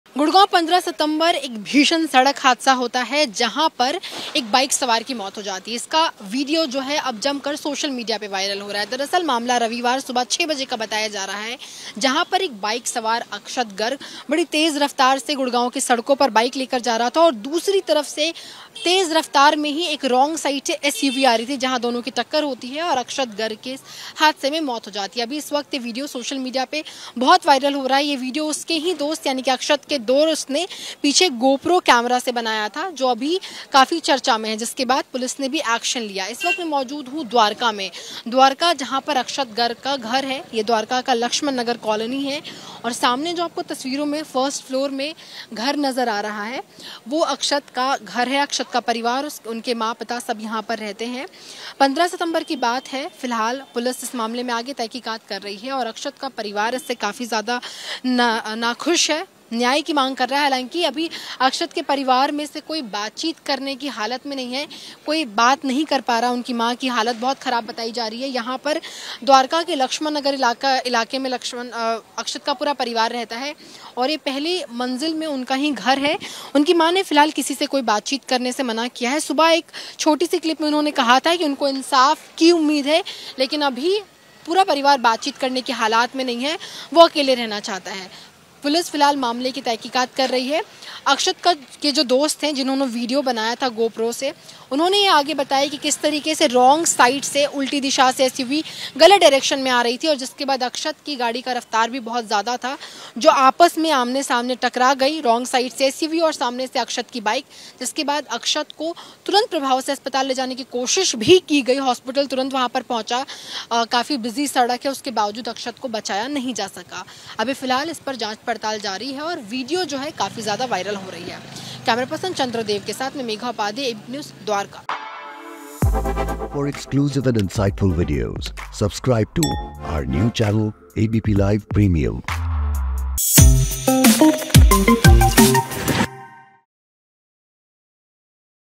Dwarka Accident : गलत दिशा में गाड़ी चलाने से गई युवक की जान, परिवार ने मांगा इंसाफ
ABP News: 15 सितंबर की सुबह करीब 6 बजे गुरुग्राम में एक भीषण सड़क हादसा हुआ, जिसमें अक्षत गर्ग नाम के एक व्यक्ति की मौत हो गई। अक्षत, जो द्वारका का रहने वाला था, तेज रफ्तार बाइक चला रहा था। सुबह के समय, जब वह जा रहा था, तभी गलत दिशा से आ रही एक एसयूवी से उसकी जोरदार टक्कर हो गई, जिससे उसकी मौके पर ही मौत हो गई। इस हादसे का वीडियो सोशल मीडिया पर तेजी से वायरल हो रहा है, जिसे अक्षत के दोस्त ने पीछे से गोप्रो कैमरे से रिकॉर्ड किया था।